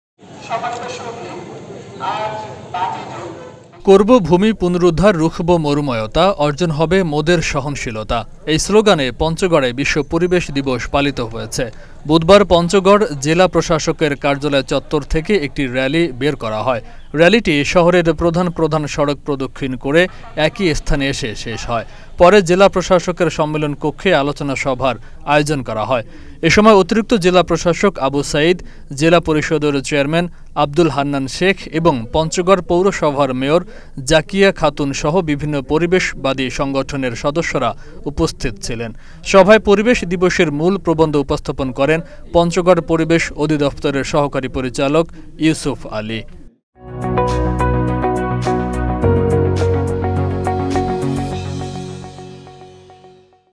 পঞ্চগড়ে বিশ্ব পরিবেশ দিবস পালিত | Channel S News | Channel S 2024
"করবো ভূমি পুনরুদ্ধার, রুখবো মরুময়তা, অর্জন হবে মোদের সহনশীলতা" এই এই স্লোগানে পঞ্চগড়ে বিশ্ব পরিবেশ দিবস পালিত হয়েছে। বুধবার পঞ্চগড় জেলা প্রশাসকের কার্যালয় চত্বর থেকে একটি র‌্যালি বের করা হয়। র‌্যালিটি শহরের প্রধান প্রধান সড়ক প্রদক্ষিণ করে একই স্থানে এসে শেষ হয়। পরে জেলা প্রশাসকের সম্মেলন কক্ষে আলোচনা সভার আয়োজন করা হয়.

1: News :
2: Entertainment News :
3: Sports News :